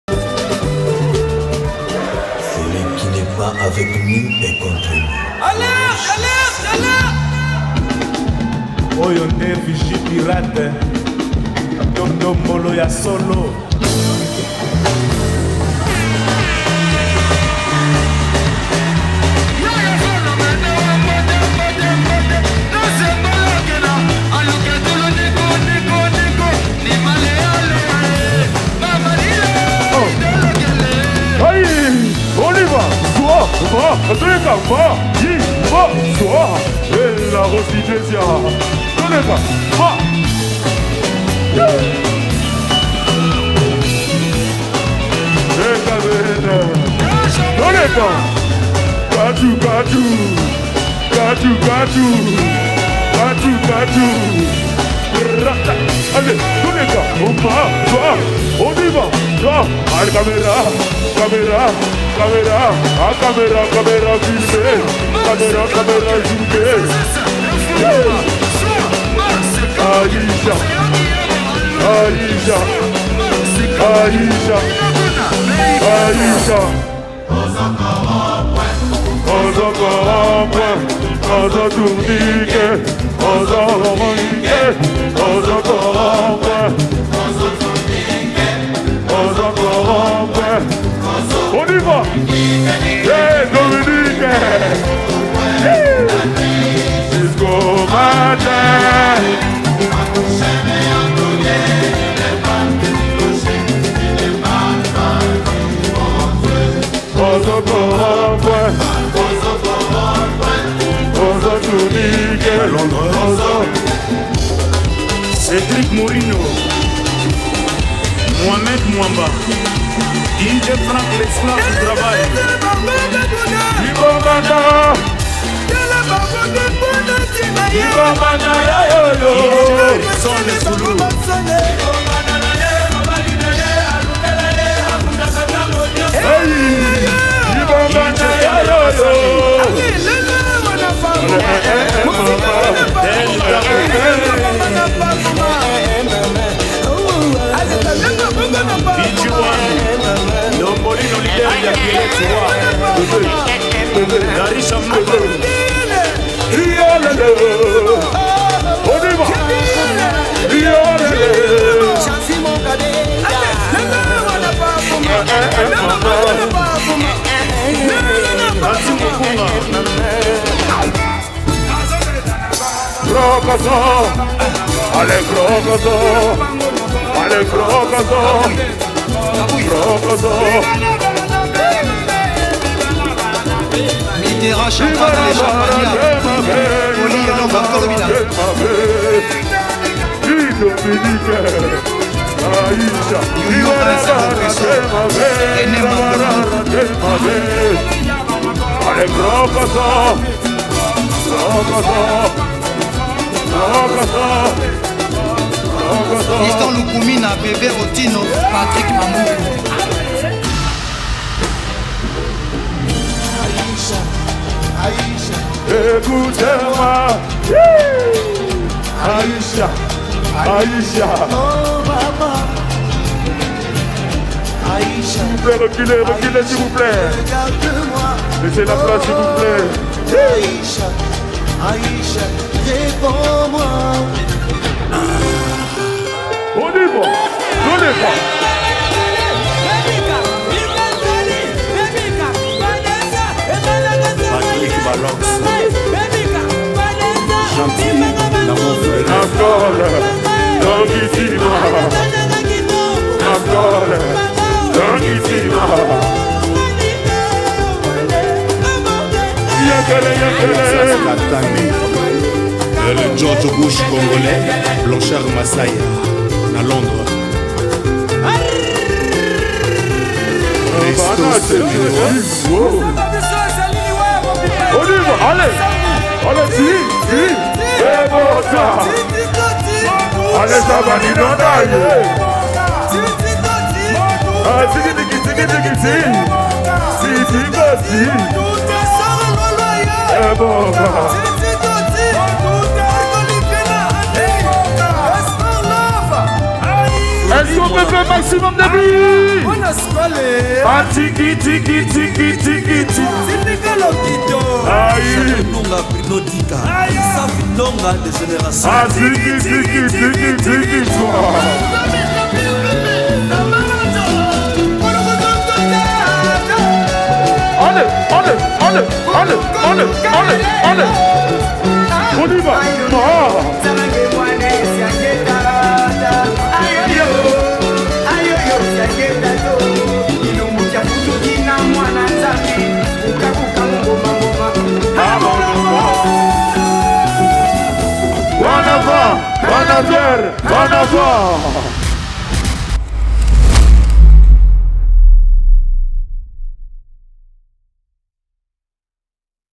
Wedding FLASHMOB Aicha & Dominique
Les Marvelous studio vous font partager cette ambiance de folie, pendant le flashmob d'Aicha & Dominique.

revivez ce moment du 20/09/2019 au palace de Villiers.